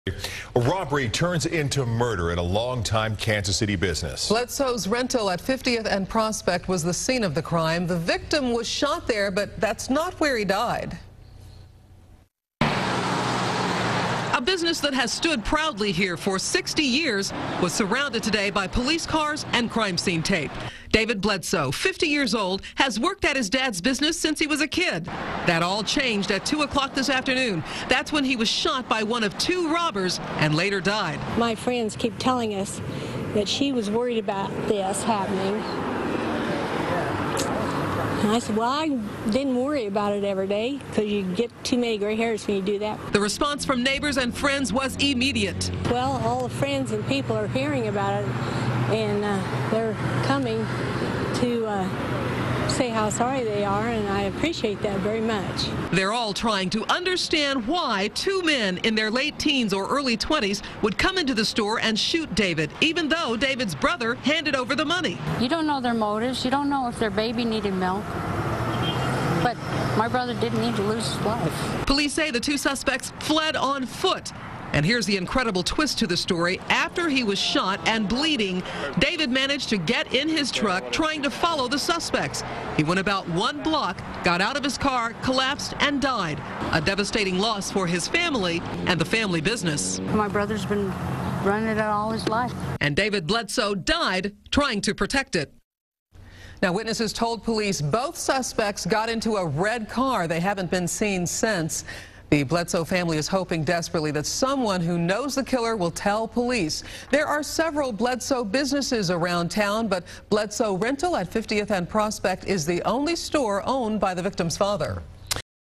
Businessman fatally shot by robbers
Robbery turns into murder at a longtime Kansas City business.